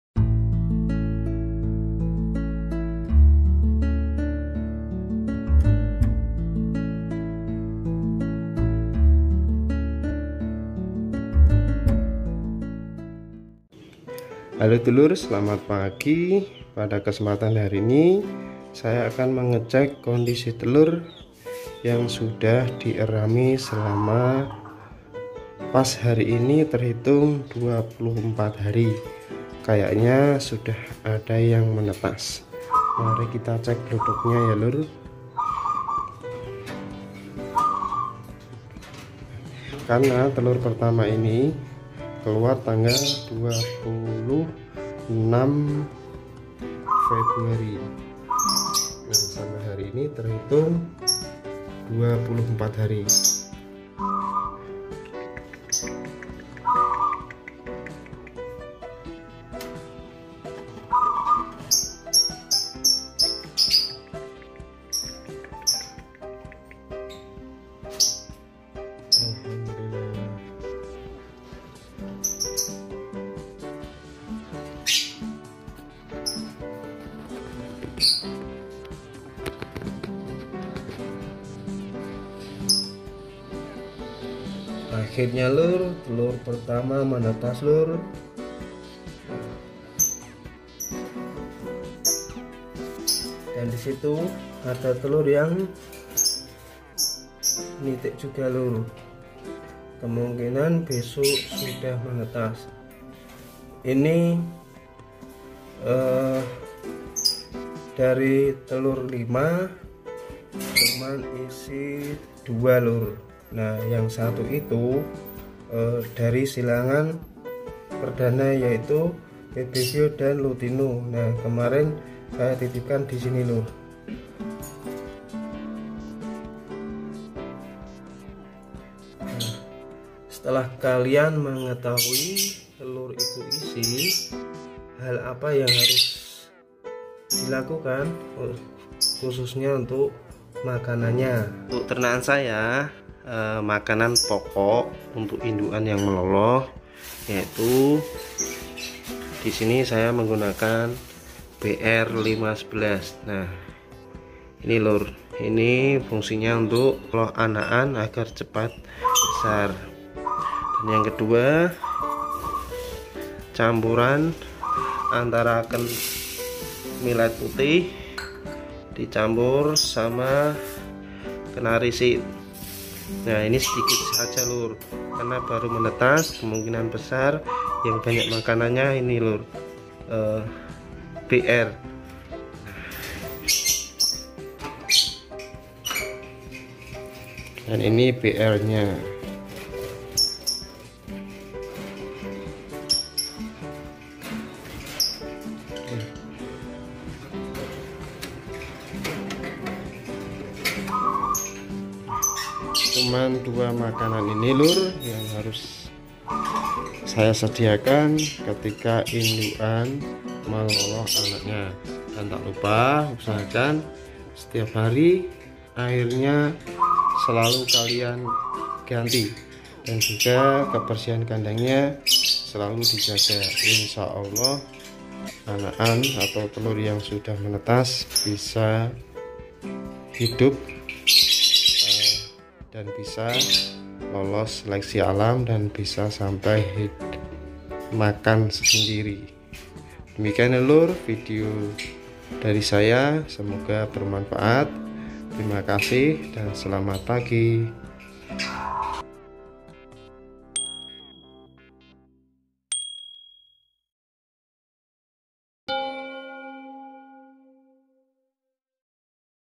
TELUR LOVEBIRD MENETAS || MAKANAN INDUKAN UNTUK MELOLOH
Pada video ini waktu yang dibutuhkan untuk mengerami telur sampai menetas yaitu 24 hari. Terhitung dari telur pertama keluar, dan makanan yang harus disediakan ketika telurnya sudah menetas.